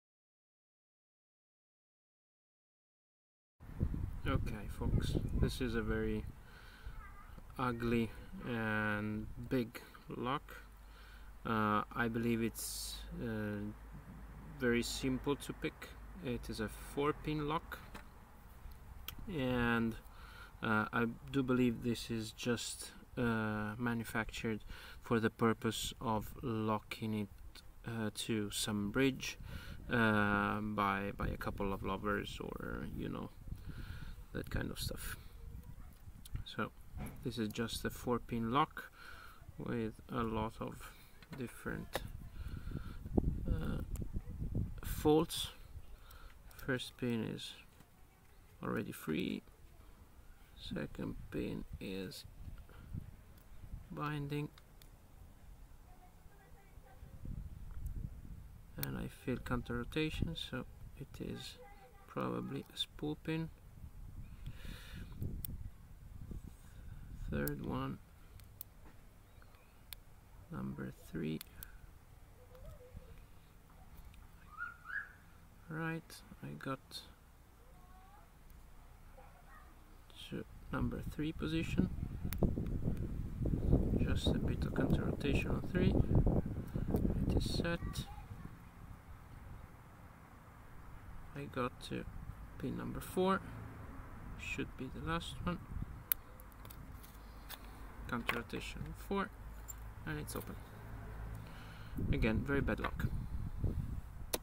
[10] Learning lock picking - 3 months experience - Lock-S love lock
Lock-S lovelock

This is a very bad 4 pins lock, with a couple of spool pins.
It has big false sets and very large clearance overall.

Very easy to pick, with a very big key way.

I wouldn't expect more than this from a love lock, but I was surprised about the spool pins inside.

This is lock 0F in my collection